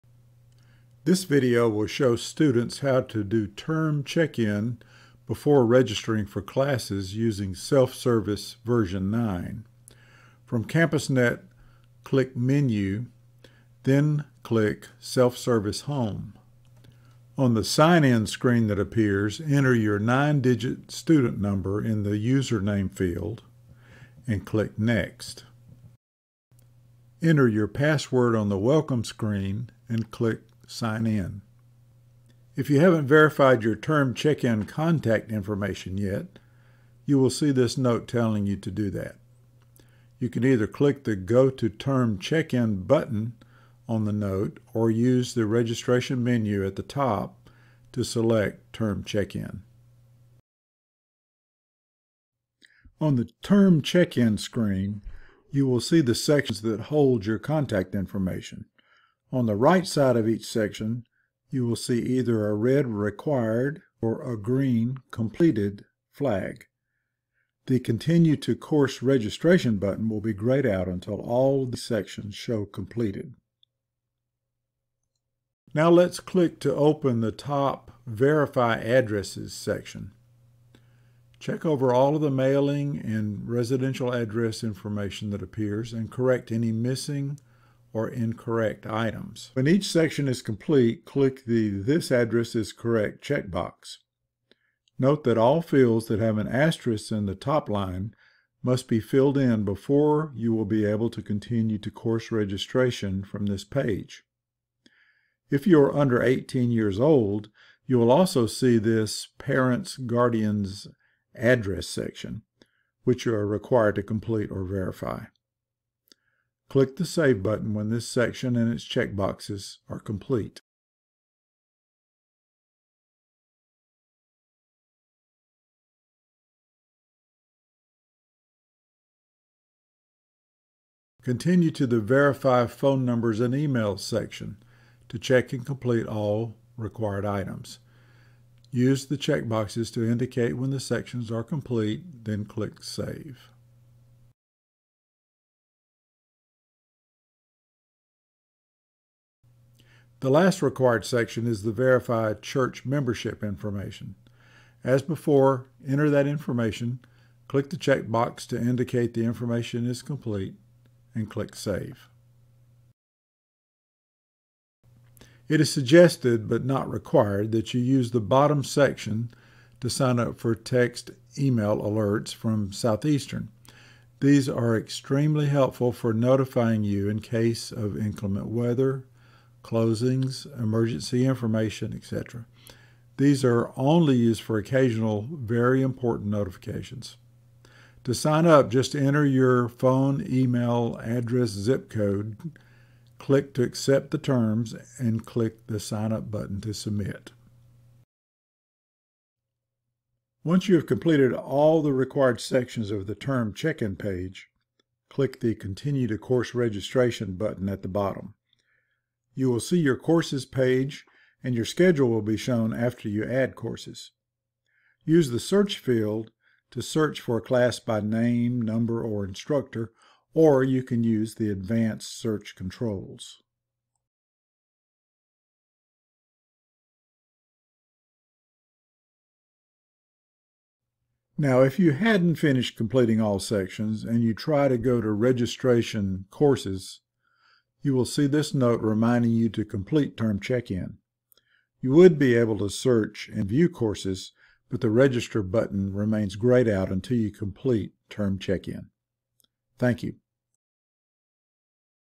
Term Check-In for Self-Service v9
Shows how to complete Term Check-In using CampusNet Self-Service v9.  Completing Term Check-In is required before you can add courses in Registration.
IMPORTANT:  After this video was made, a new Term Check-In REQUIRED section was added for managing the release of your student records (FERPA requirement).  For details on setting up this section, see this article: